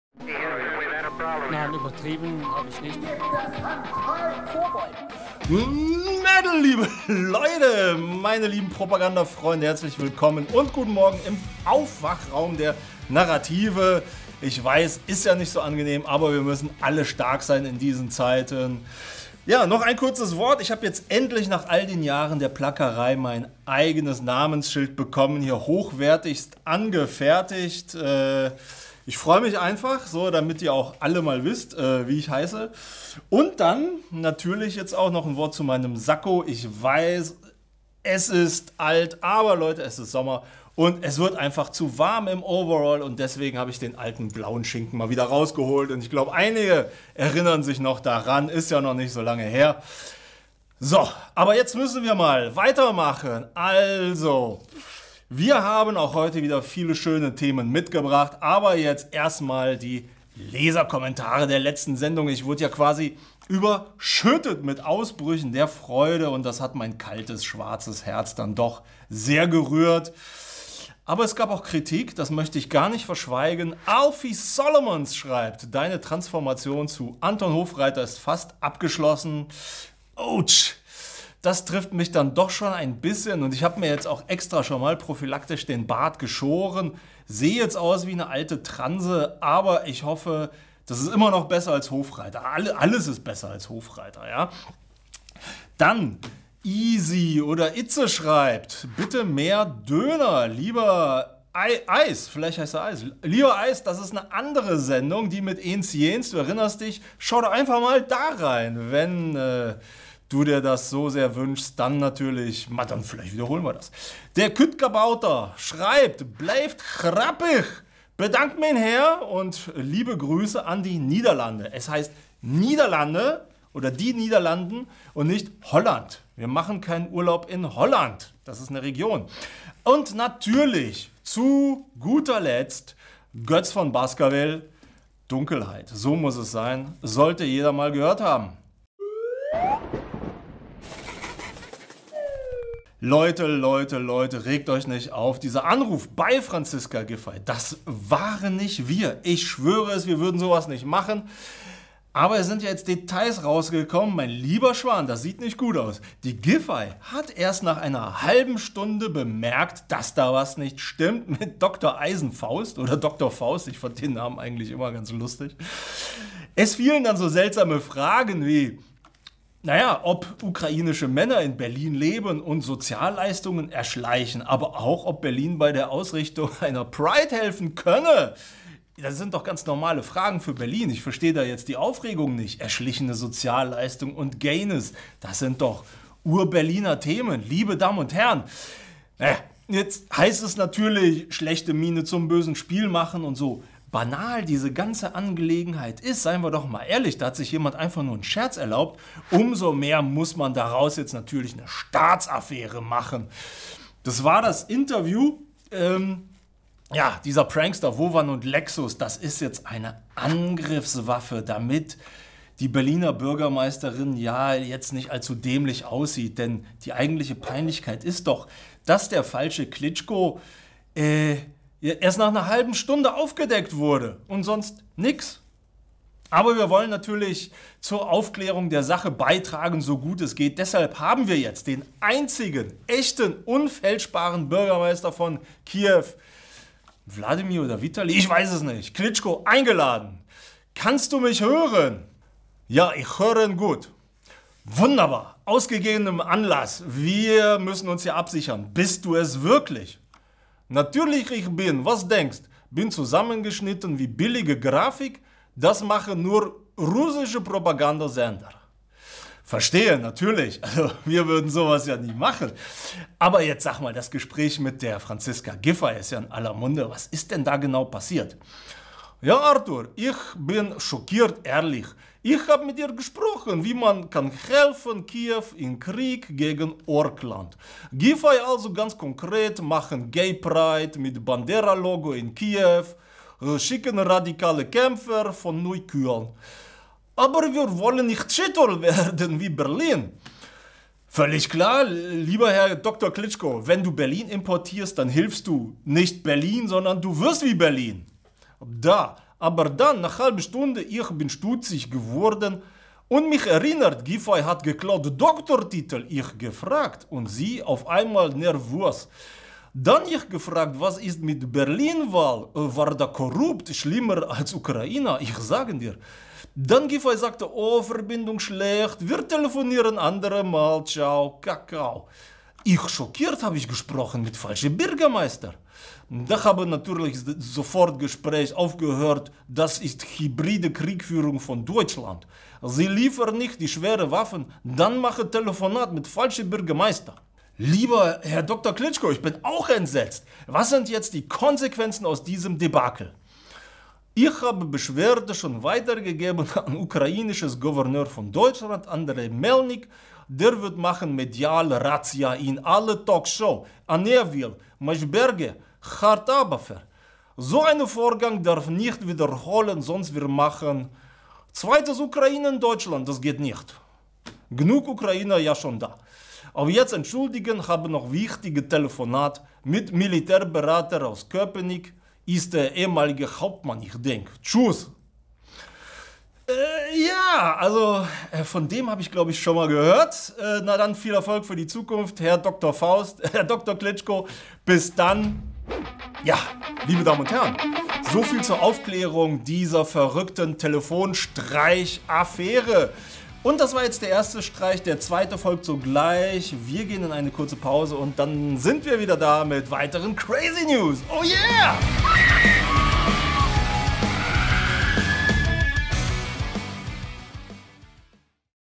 Skandal-Interview mit FAKE-Bürgermeister*IN - InfraRot Narrativschlaf
Das war schon ein Ding mit dem Klitschko. Erst ruft er die Giffey an und dann heißt es, dass das war ein Fake-Telefonat. Ich denke, Klitschko hat zu viele Fragen über den Doktortitel von Franziska gestellt. Naja, bei uns erfahrt ihr die gaaaanze Wahrheit!
Liebe Grüße,
Arthuro
🔵Hier könnt ihr uns unterstützen🔵